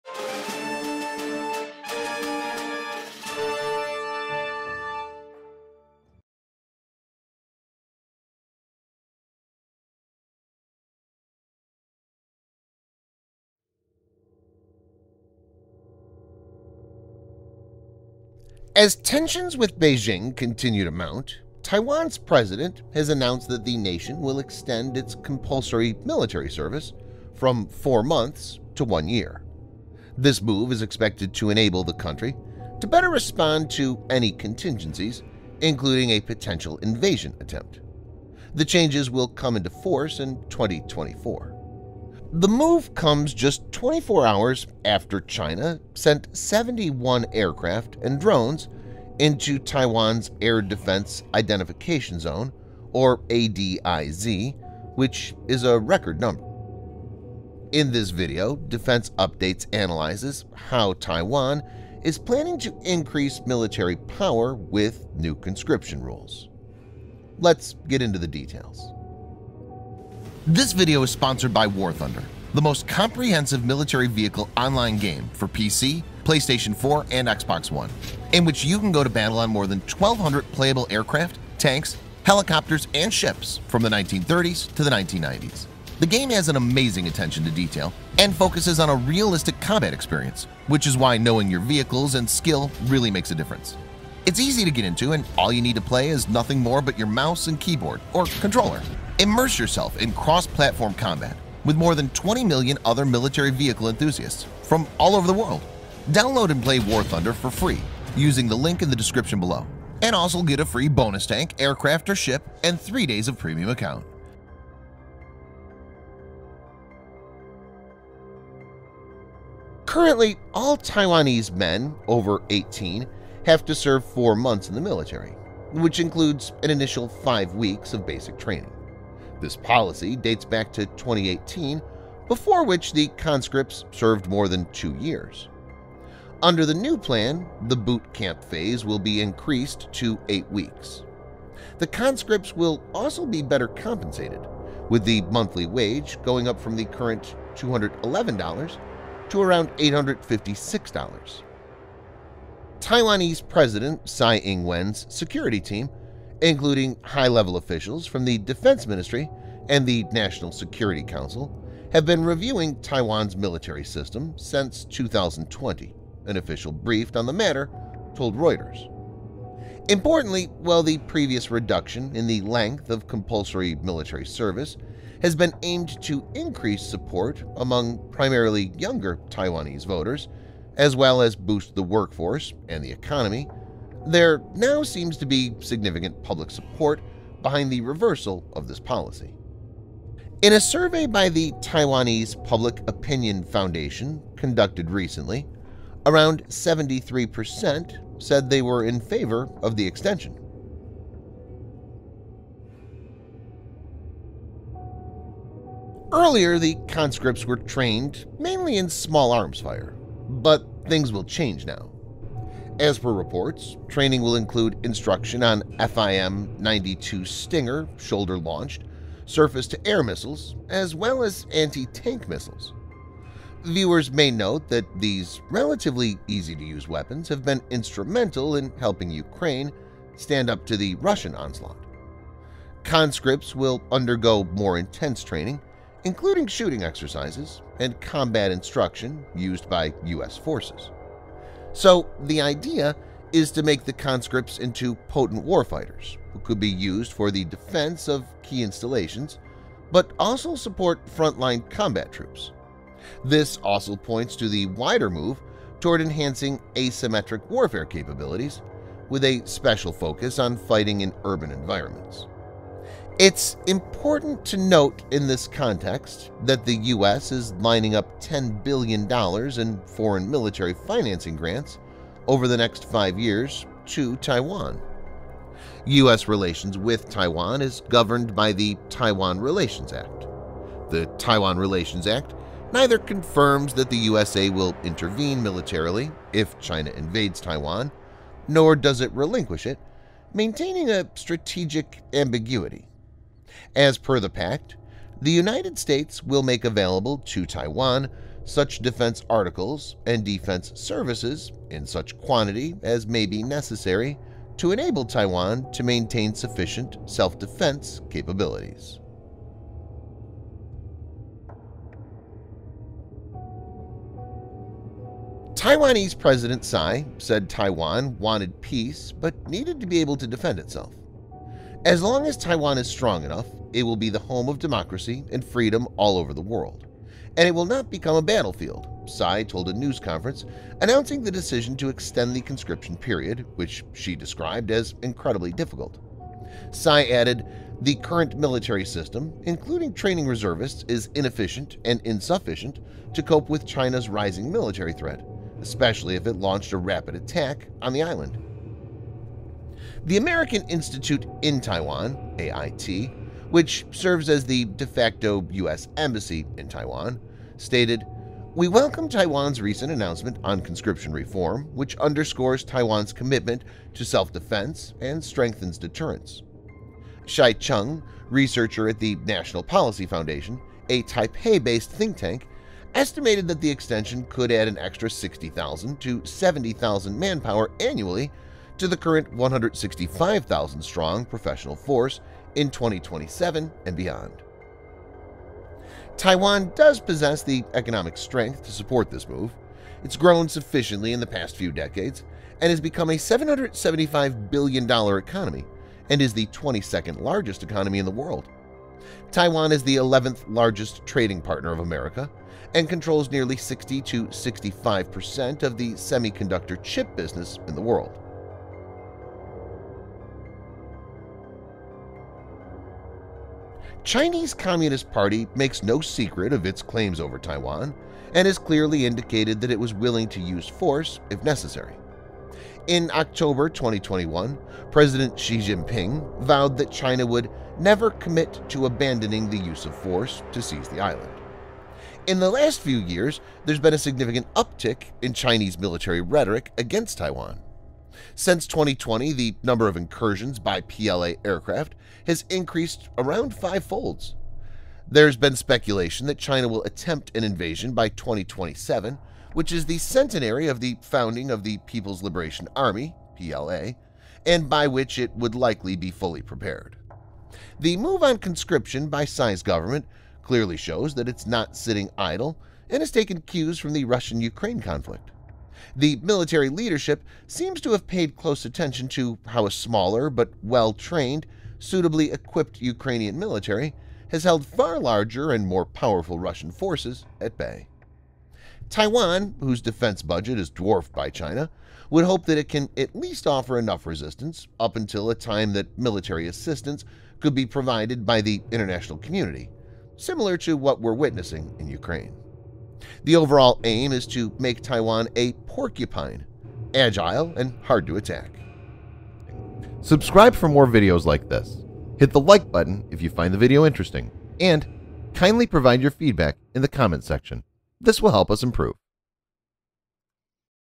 Taiwan extend its compulsory military service !
As tensions with Beijing continue to mount, Taiwan’s president has announced that the nation will extend its compulsory military service from four months to one year.
This move is expected to enable the country to better respond to any contingencies, including a potential invasion attempt.

The changes will come into force in 2024.

The move comes just 24 hours after China sent 71 aircraft and drones into Taiwan’s Air Defense Identification Zone, or ADIZ which is a record number.

In this video Defense Updates analyzes how Taiwan is planning to increase military power with new conscription rules ?

BACKGROUND MUSIC

IMAGES USES